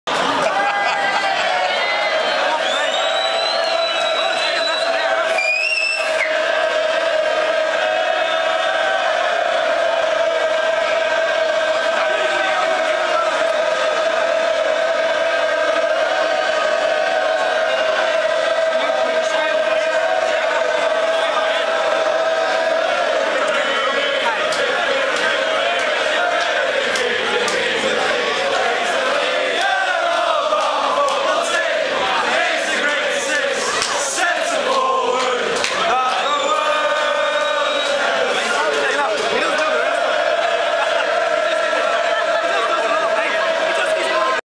Mr Miyagi
Mr Miyagi in Bishp Blaize after 2:0 win agains Bolton